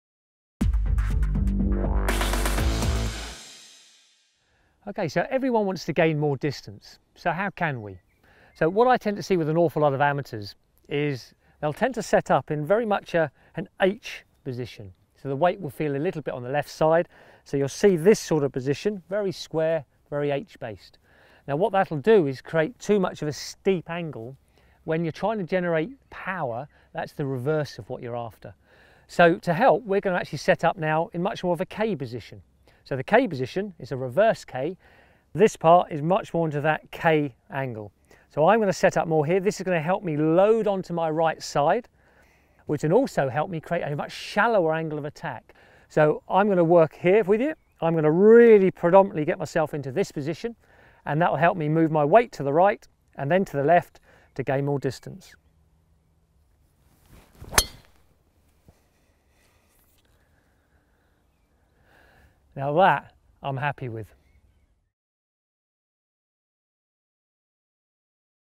Golf tips: Increase your distance | GolfMagic.com
- How to hit the ball further, with former European Tour pro Carl Watts.